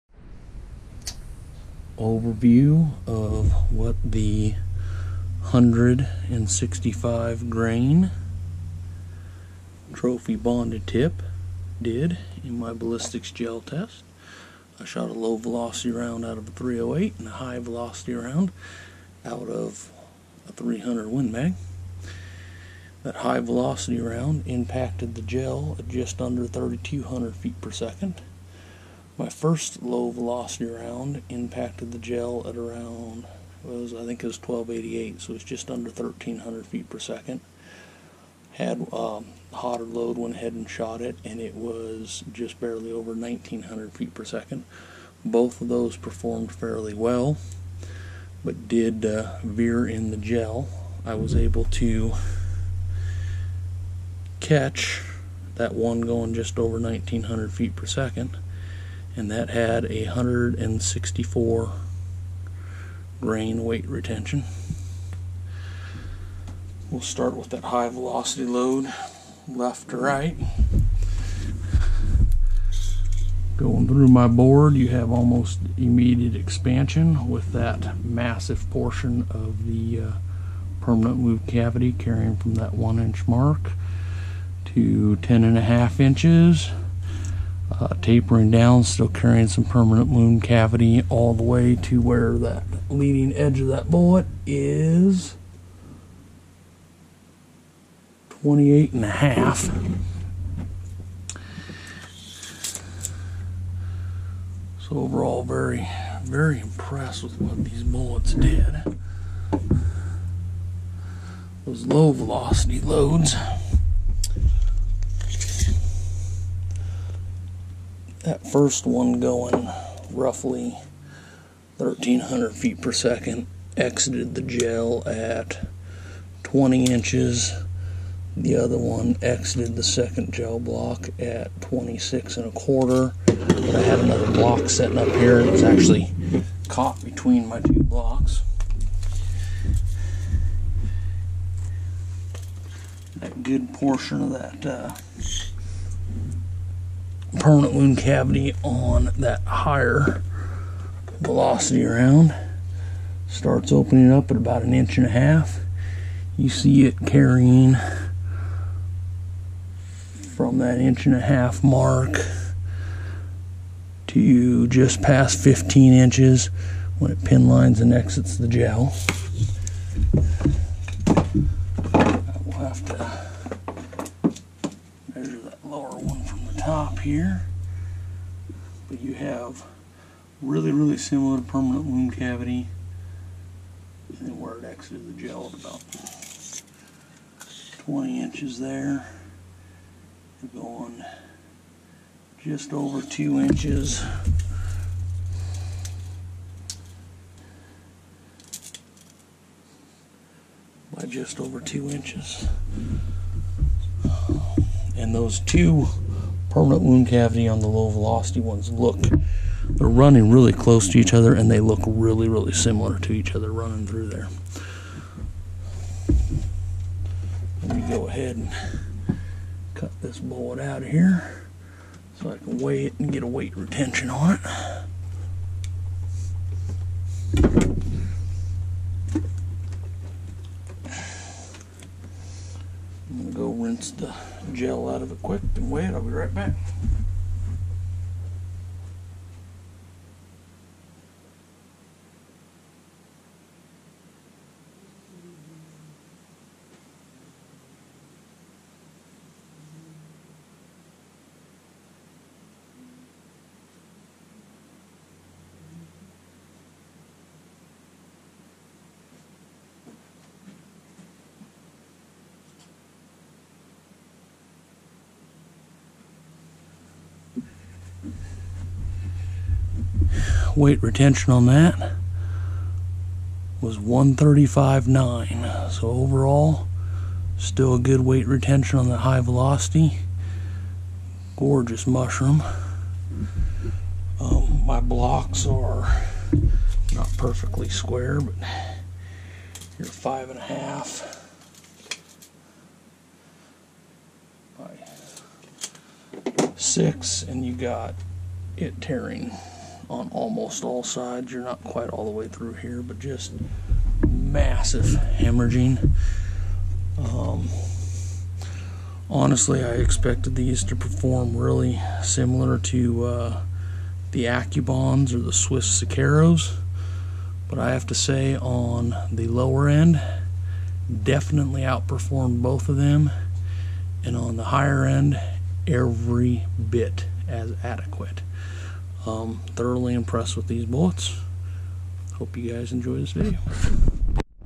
Federal Premium Trophy Bonded Tip 165 gr 30 cal ballistics gel block overview
Going over how the 165 gr 30 cal Federal Premium Trophy Bonded Tip performed in 10% ballistics gelatin.

 I have to say I am more than impressed with the ballistics performance of these bullets. I thought they would be more similar in performance to the Accubond or Scirocco than they are. In my opinion they out performed them all the way around especially on the low end. I would have to say ballistically these and the Nosler ABLR have the broadest effective velocity ranges and are definitely going to be one I will be using on game.